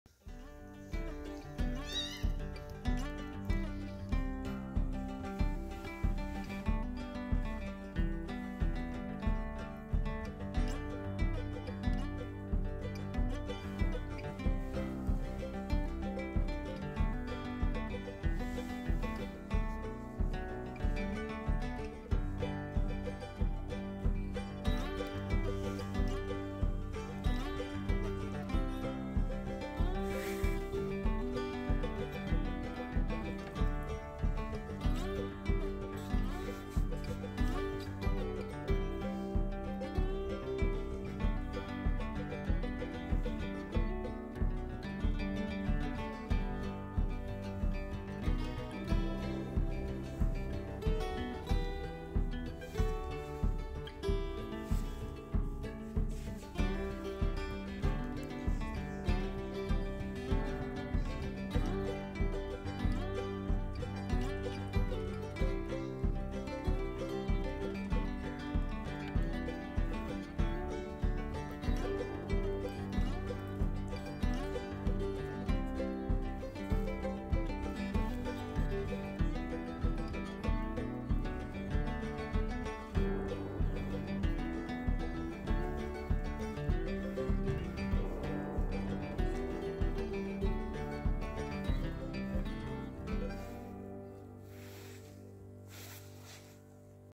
She is biting my finger for a reason 😂
My kitten smells  nice food smell, so she licks and try to eat my finger 😂